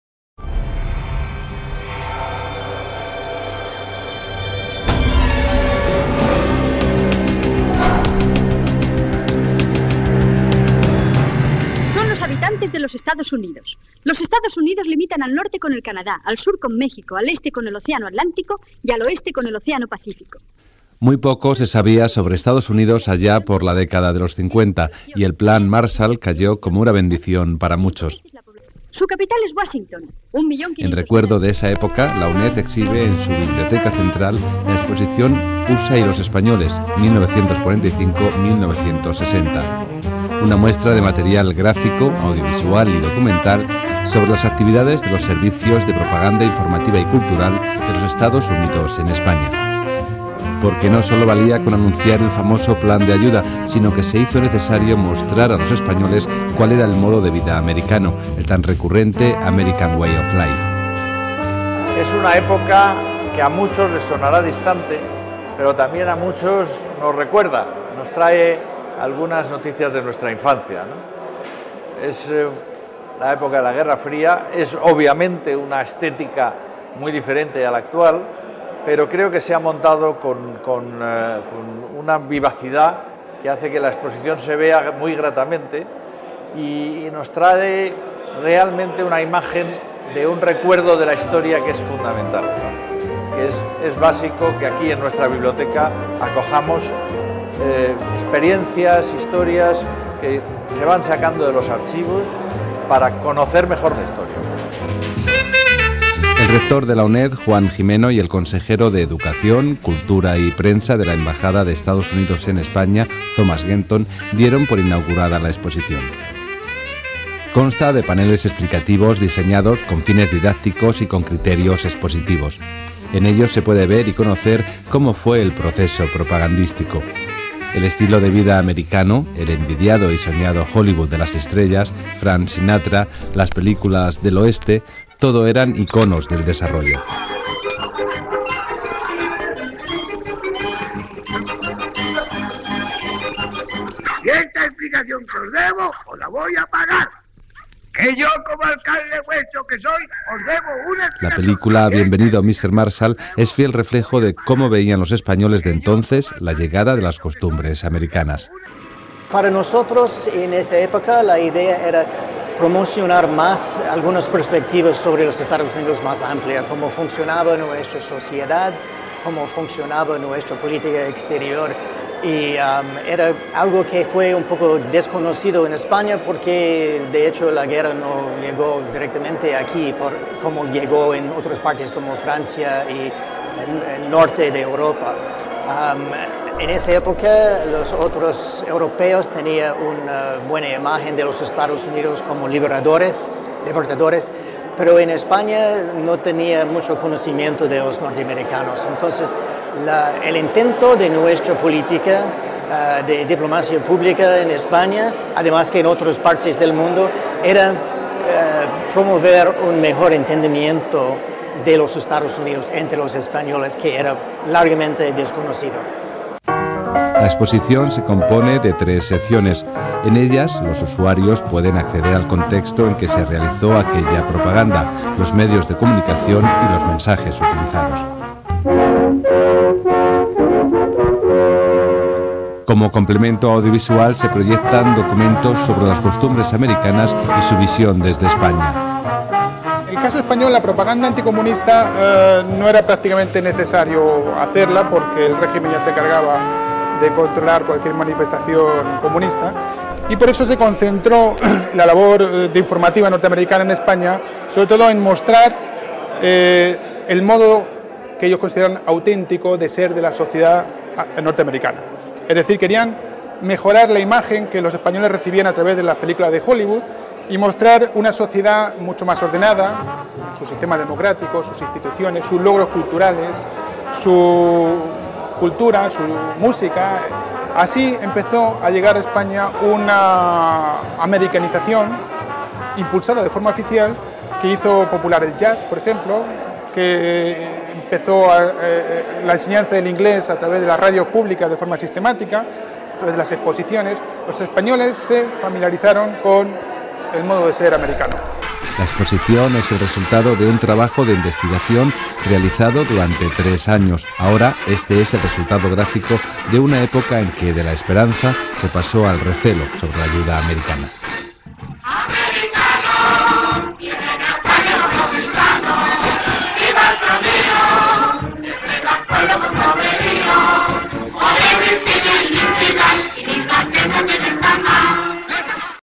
18-06-2010. PDA: Exposición USA - España.
Muy poco se sabía sobre Estados Unidos allá por la década de los 50, y el Plan Marshall cayó como una bendición para muchos. En recuerdo de esa época, la UNED exhibe en su biblioteca central la exposición "USA y los Españoles, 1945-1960", una muestra de material gráfico, audiovisual y documental sobre las actividades de los servicios de propaganda informativa y cultural de los Estados Unidos en España. Porque no sólo valía con anunciar el famoso plan de ayuda, sino que se hizo necesario mostrar a los españoles cuál era el modo de vida americano, el tan recurrente "American Way of Life". Consta de paneles explicativos diseñados con fines didácticos y con criterios expositivos. En ellos se puede ver y conocer cómo fue el proceso propagandístico. Para saber más: